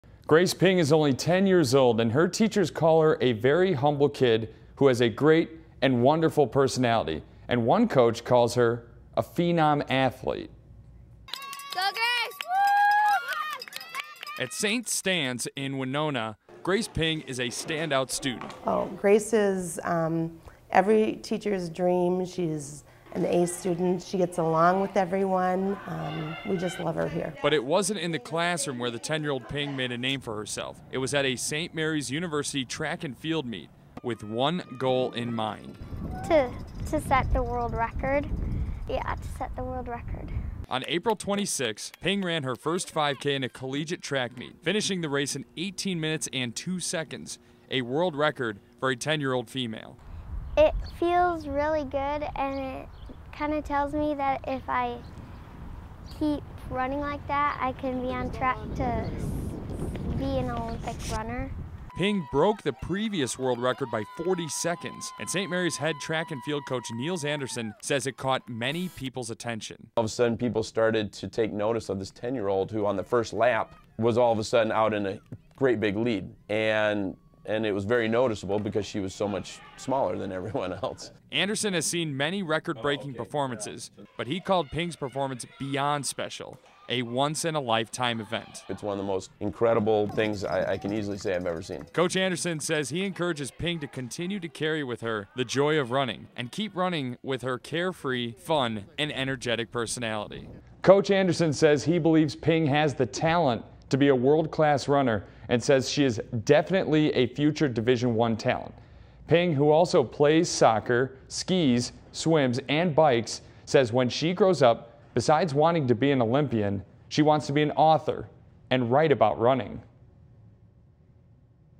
Grace Ping Story
Sean Tehan's reporting, photography reel (all reporting, producing, photography, and editing completed independently)